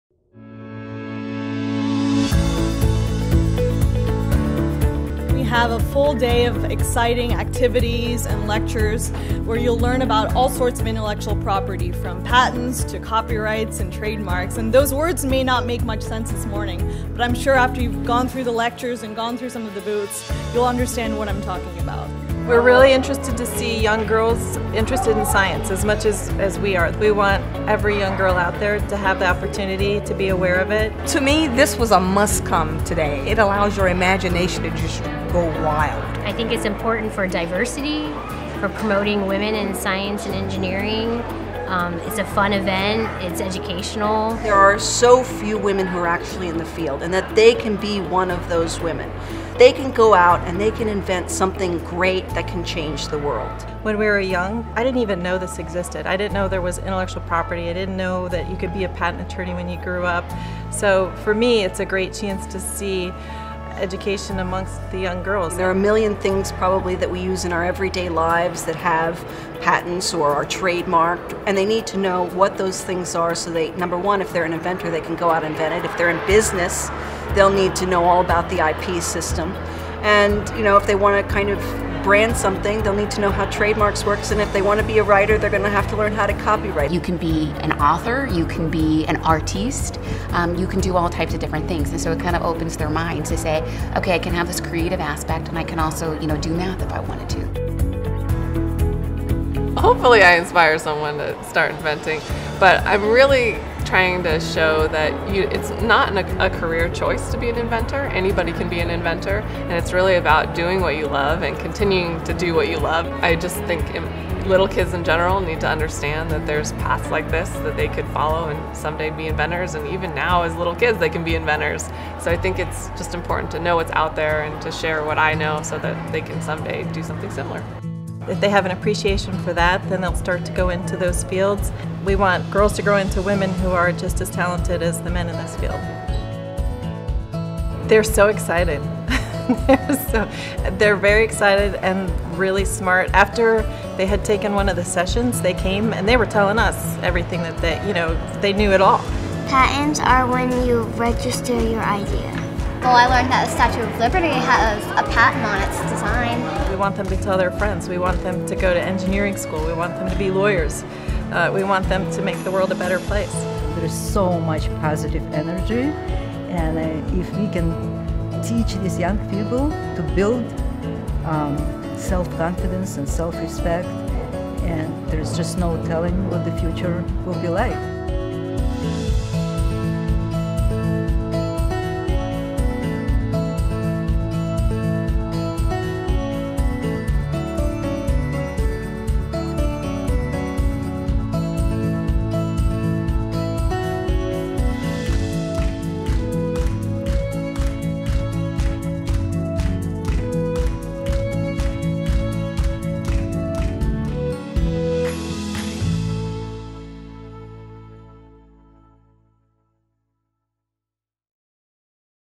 IP Patch Video
IPO Education Foundation and the IPO Women in IP Committee hosted events to teach local Girl Scouts about intellectual property through the IP Patch!